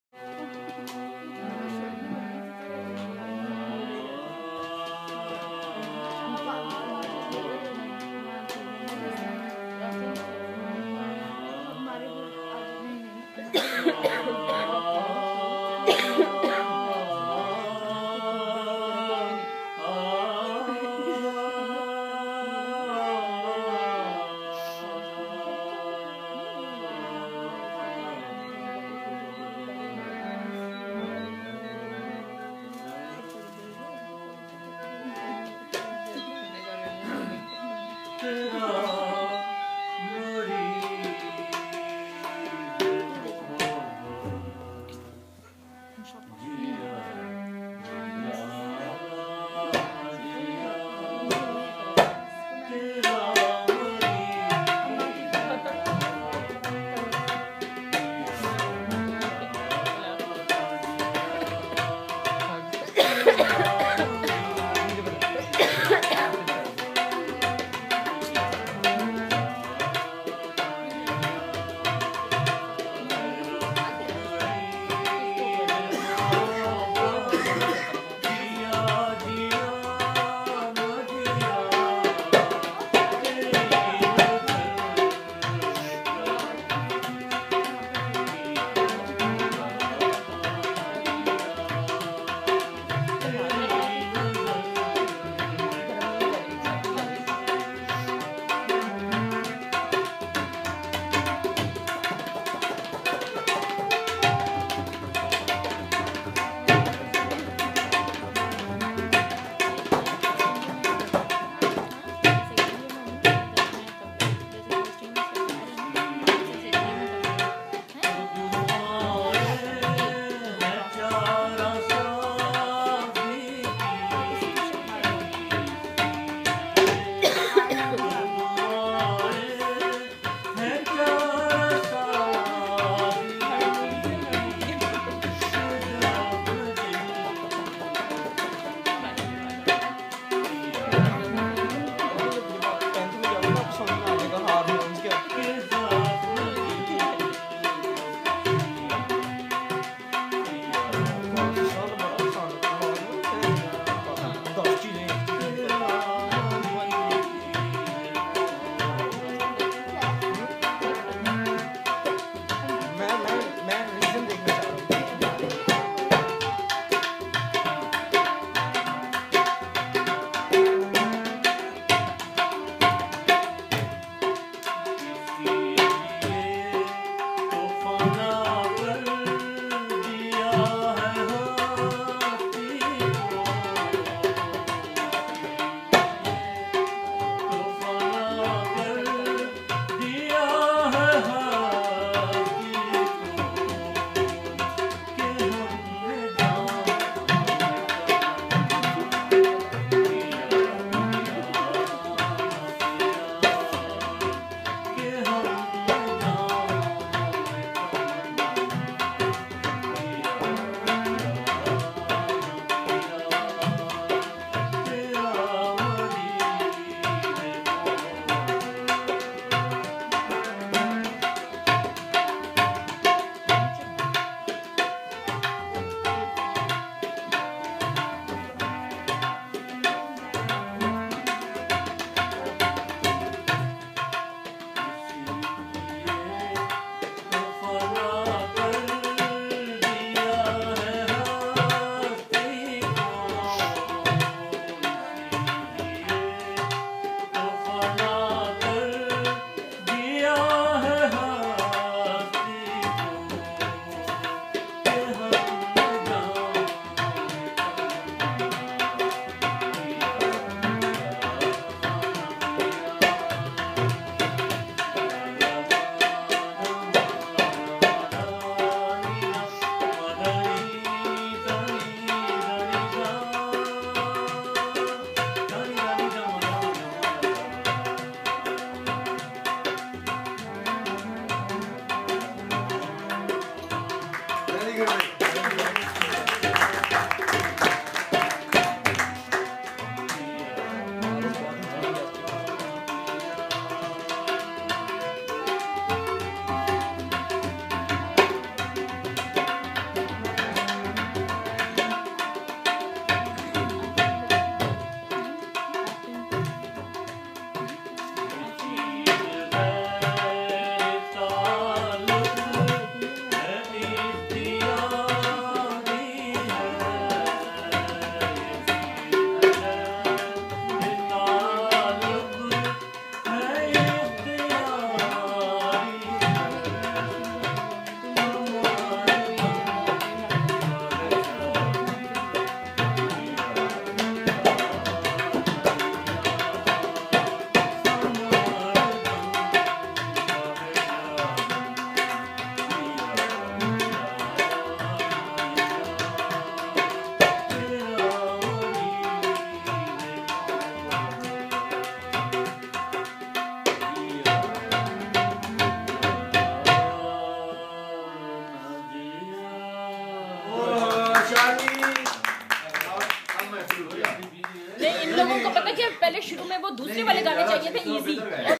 Shah Mujeeb
Shah Mujeeb ud Din singing in Lahore December 2012